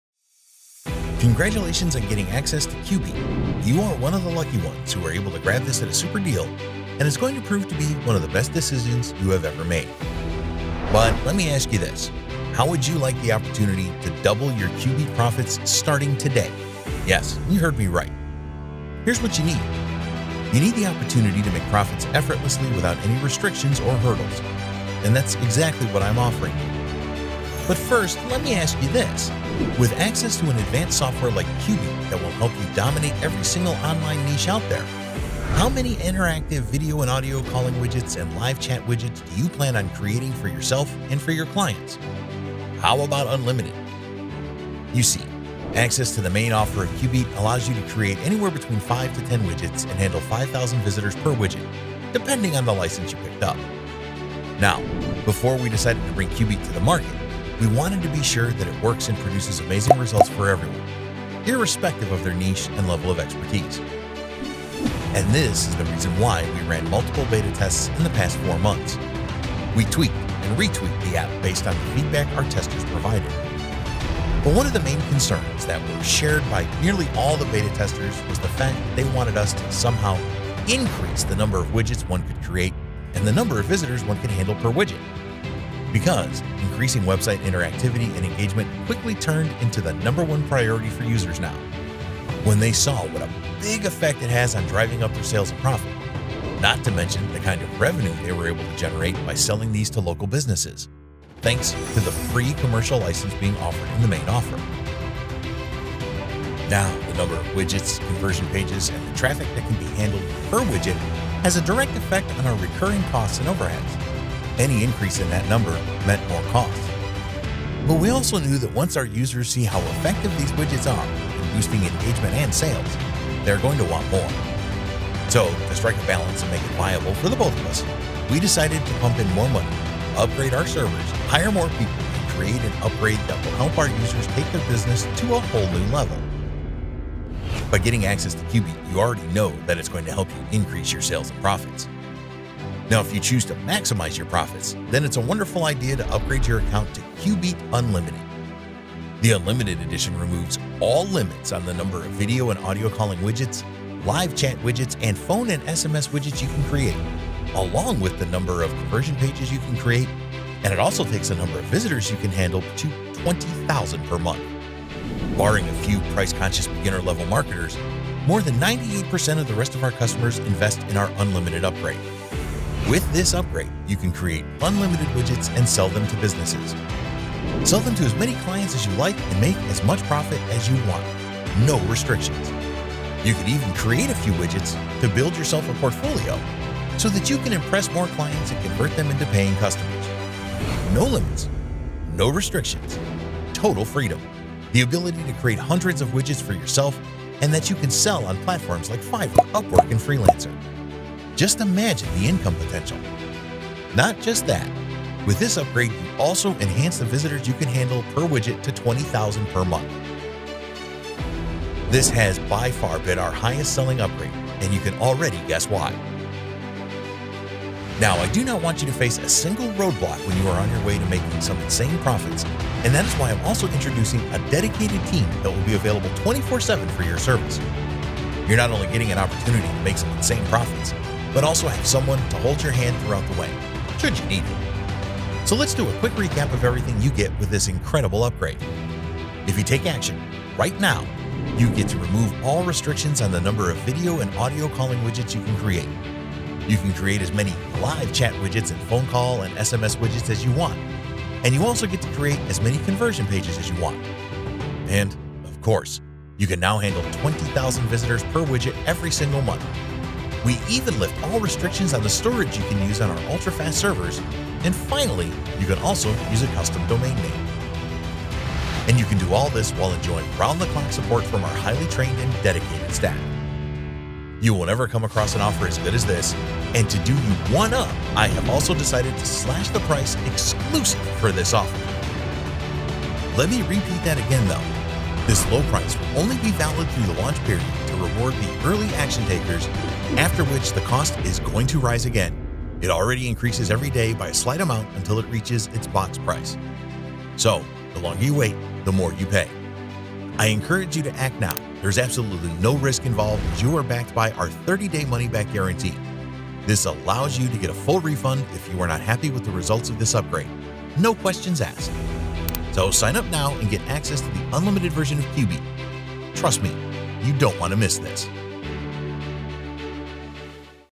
Cubeet OTO Upsells 1 - Cubeet Unlimited + LIVE Video Calling Tool - Cubeet Bundle Software Review
Cubeet OTO 1: Unlimited + LIVE Video Calling Tool

Create and sell UNLIMITED interactive 2-way video/audio calling widgets, conversion pages & more.

Recap everything you will get:

[+]   2-way live video calling

[+]   Create unlimited widgets with 20k/mo traffic capability

[+]   Create unlimited flows

[+]   Create unlimited instant conversion pages

[+]   Unlimited asset storage capacity

[+]   Support custom domain

[+]   Commercial license included